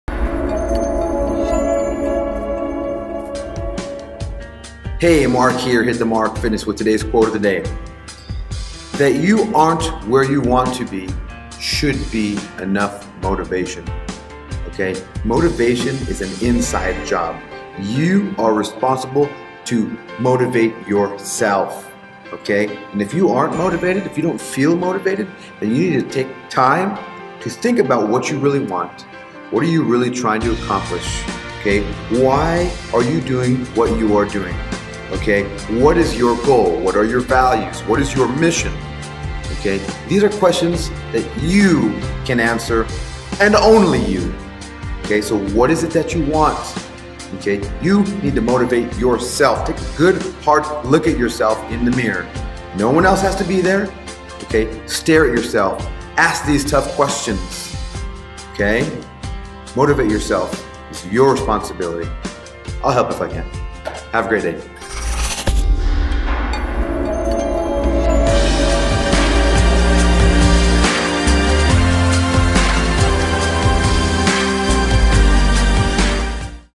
What do you really want?
Be on a mission. Have a vision for your life? What do you want? Take a good hard look. Ask yourself the tough questions.